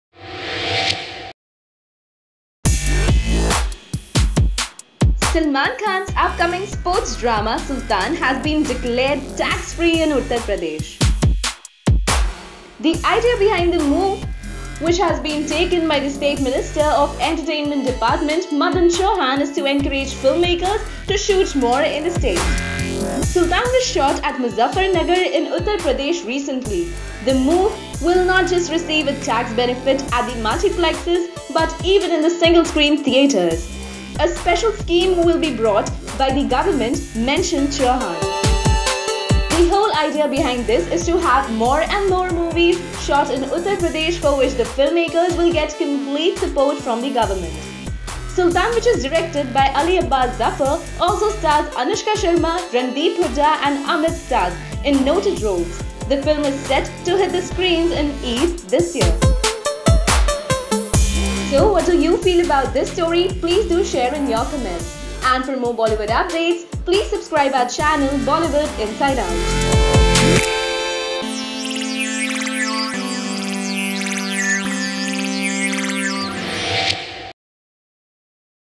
Salman Khan: Sultan Declared TAX FREE In Uttar Pradesh | Bollywood Inside Out
Since Salman Khan and UP Chief Minister Akhilesh Yadav now share an amazing rapport, looks like things are clearly working well for the makers of Sultan. Last month Salman was shooting in Muzaffarnagar, UP for the film and the CM had ensured the best security arrangement for the superstar. The actor, along with director Ali Abbas Zafar had thanked the CM on Twitter for taking such good care of them. Now we hear that the film, which is still not complete, is going to be made tax-free in UP. Madan Chauhan, the State Minister of Entertainment Department UP, recently announced the same. The UP government wants to encourage producers to shoot in the state and Sultan has clearly won the first move advantage. Check it out..